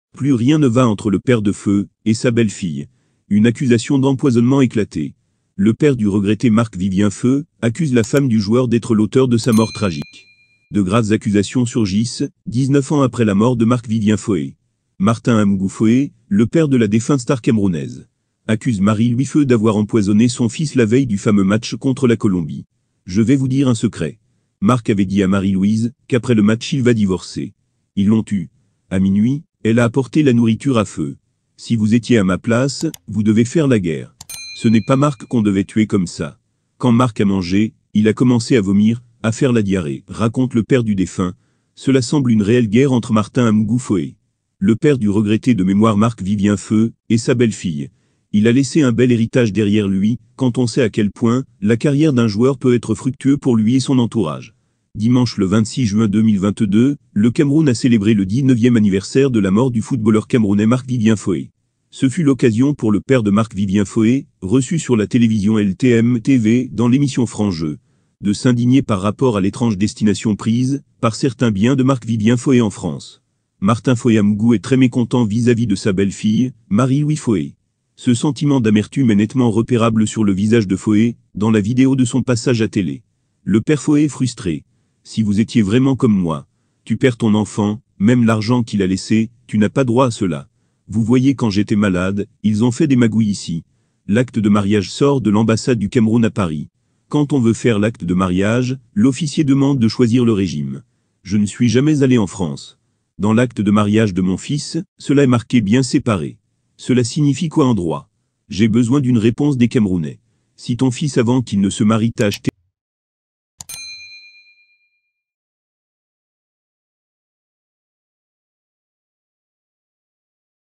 lourdes accusations du père de Marc Vivien Foe contre son épouse 19 ans après
Le père de Marc Vivien Foe accuse sa femme d'être à l'origine